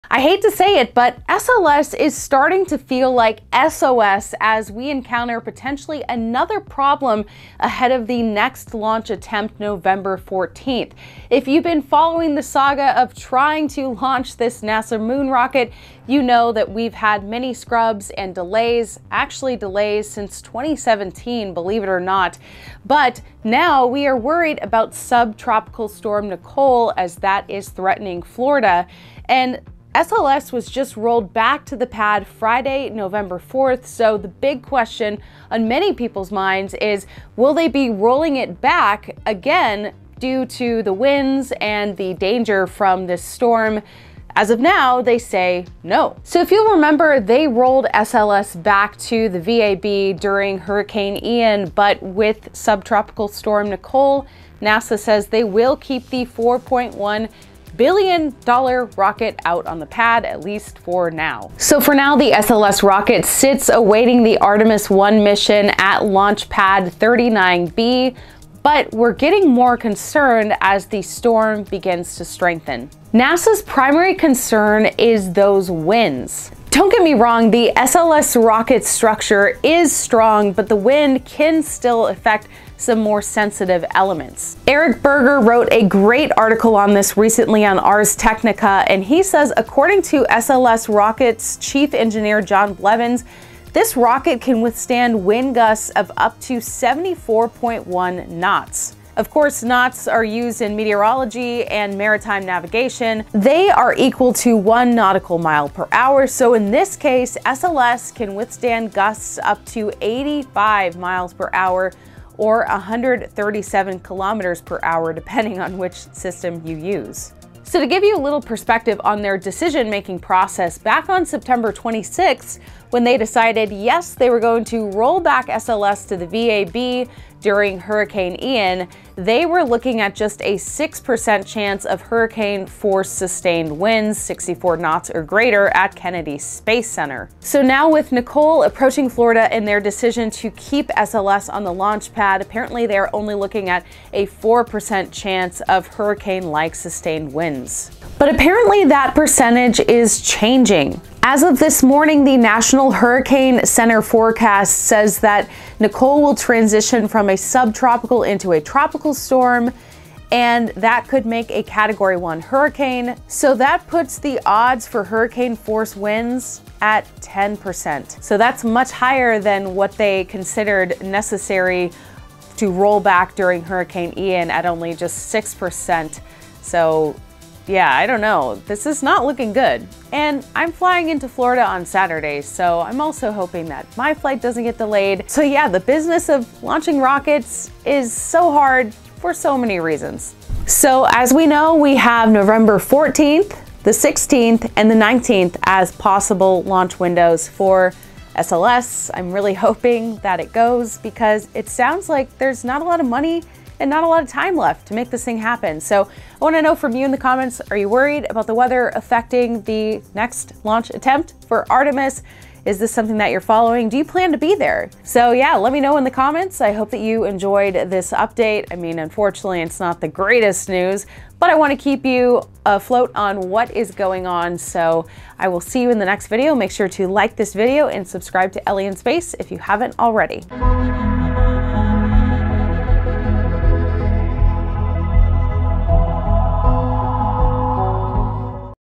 SLS or SOS? NASA is running out of time and money to launch Artemis
LAUNCH NOW PUSHED TO NOVEMBER 16

To read Eric Berger's article that I have cited, head

For now, NASA will leave it's behemoth rocket out on the pad ahead of the next launch attempt. But there's trouble approaching with storm Nicole.